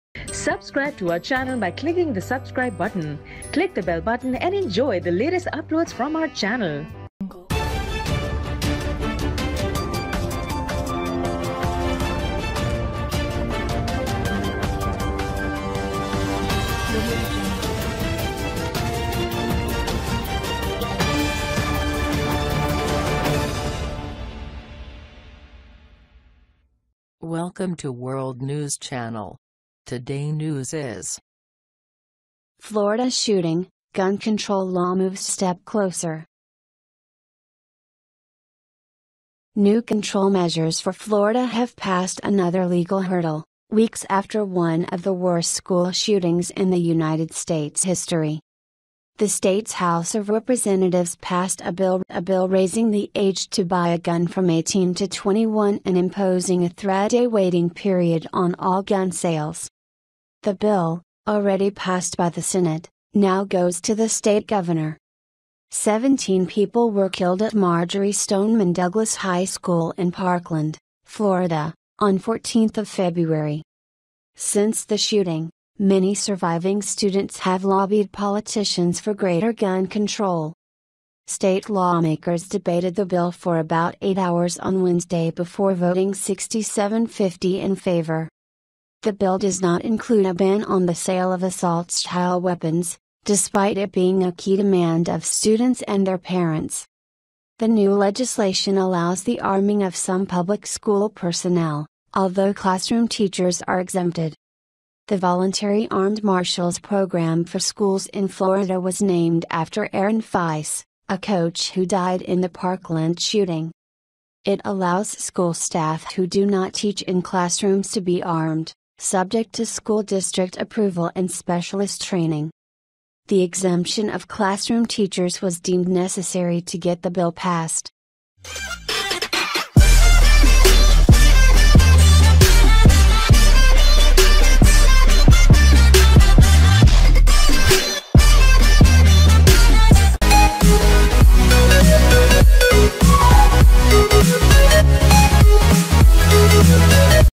Florida shooting Gun control law moves step closer
New gun control measures for Florida have passed another legal hurdle, weeks after one of the worst school shootings in US history.

The state's House of Representatives passed a bill raising the age to buy a gun from 18 to 21 and imposing a three-day waiting period on all gun sales.

The bill, already passed by the Senate, now goes to the state governor.

Seventeen people were killed at Marjory Stoneman Douglas High School in Parkland, Florida, on 14 February.

One mother, two school shootings
Trump clashes with Republicans on guns
Guns banned on dating app Bumble
Expelled former student Nikolas Cruz, 19, has been charged in the attack. He was formally indicted by a grand jury on Wednesday on 34 counts, including 17 counts of premeditated first-degree murder and 17 counts of attempted first-degree murder.

He is suspected of using a legally bought AR-15 semi-automatic rifle to carry out the 10-minute attack, gunning down teachers and students.

What is in the new law?
In addition to raising the age and bringing in the three-day waiting period, the legislation:

Introduces a voluntary armed "guardian programme" for schools, named after Aaron Feis, a coach who died in the Parkland shooting. It allows school personnel to be armed, subject to school district approval and specialist training
Classroom teachers are excluded from carrying arms unless they have a security forces background
Bans devices, such as bump stocks, that modify a semi-automatic weapon to fully automatic
Raises mental health funding and increases the power to seize or ban guns under mental health concerns
The legislation does not include a ban on the sale of assault-style weapons like the AR-15, despite it being a key demand of Parkland students and their parents.

Florida law already mandates a three-day waiting period for the purchase of a handgun but a person as young as 18 can buy a rifle with no waiting period.

The Republican-controlled House debated the bill for about eight hours on Wednesday before voting 67-50 in favour.

Has the move been welcomed?
Ryan Petty, whose daughter was among those killed, hailed the House's vote in a tweet.

It passed!  Thanks to everyone that supported us, the families in making sure we are the last families that have to experience a tragedy like this

He said: "We know that when it comes to preventing future acts of school violence, today's vote is just the beginning of our journey."

State Democratic Party legislators who backed the bill said it was necessary despite the "poison pill" of allowing more guns in schools under the guardian programme. Florida now joins at least six other states in allowing school employees to carry firearms.

Following the Parkland shooting, many surviving students had lobbied politicians for greater gun control.

Gun control activists will hope the Florida measures will be a turning point in their battle with the powerful National Rifle Association and others who defend firearms ownership under the Second Amendment of the US constitution.

On the day the vote passed, a 17-year-old woman student was killed in a suspected accidental shooting at a high school in Alabama.

What happens now?
Governor Rick Scott will need to sign the bill, known as the Marjory Stoneman Douglas High School Public Safety Act, into law.

It will automatically become law within 15 days unless he vetoes it.

Mr Scott has not yet said he will back it, telling reporters he would "review the bill line by line" and consult victims' families.

He has previously said he opposes US President Donald Trump's call to arm teachers. He had also previously opposed increasing the purchase age from 18 to 21.